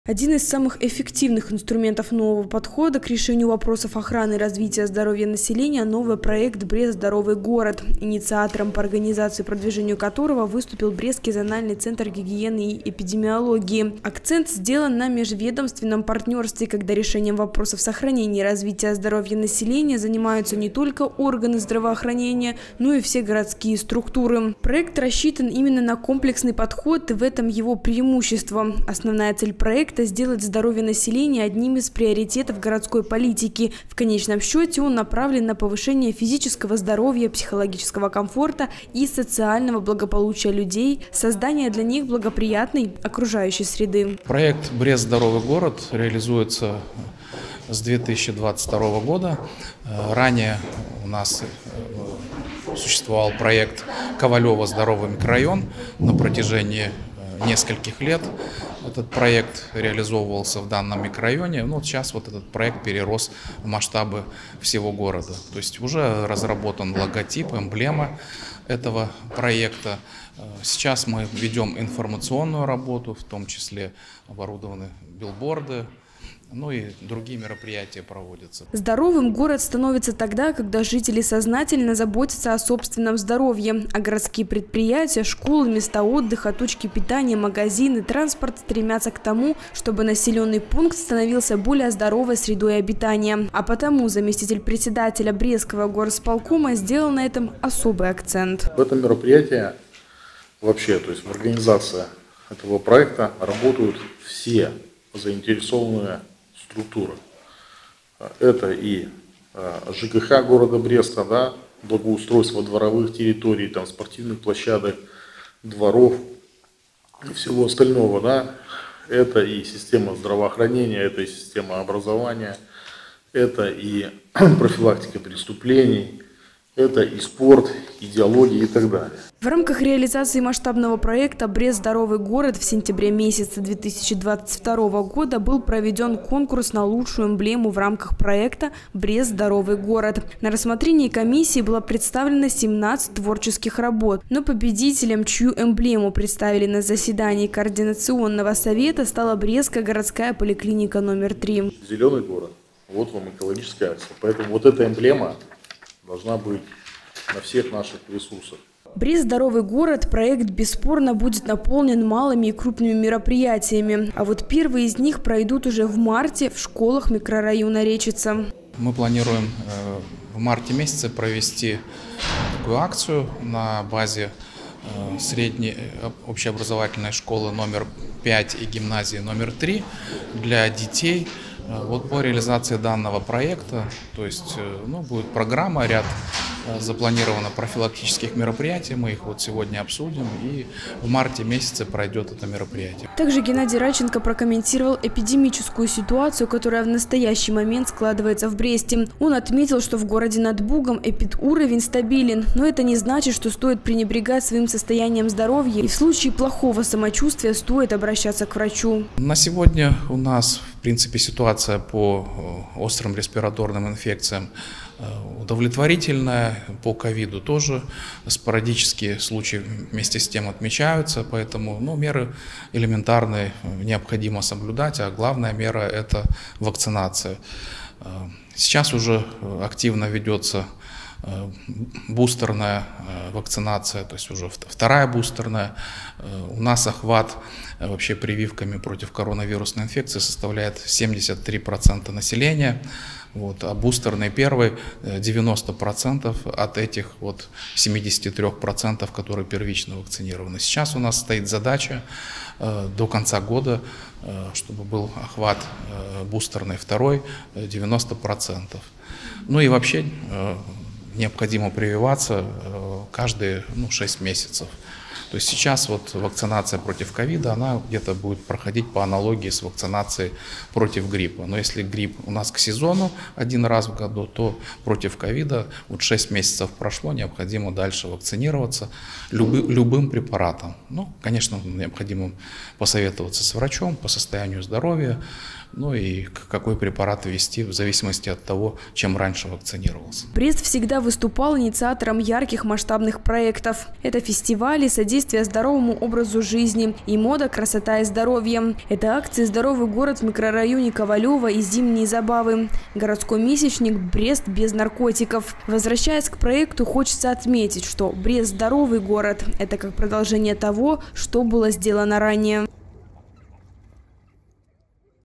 2023-03-03 г. Брест. Координационный Совет по реализации проекта. Новости на Буг-ТВ.
Координационный Совет по реализации проекта «Брест – здоровый город».
Здоровый город – это место, где постоянно развивается физическая и социальная среда, организовываются ресурсы таким образом, чтобы люди помогали друг другу в улучшении жизни и раскрытии своих возможностей. Проект предусматривает объединение усилий органов власти, деловых кругов, общественных объединений и непосредственно самих жителей ради укрепления здоровья населения. Такая стратегия разработана и будет реализована в городе над Бугом.  Масштабный проектс говорящим названием «Брест – здоровый город» накануне обсуждали на координационном совете в горисполкоме.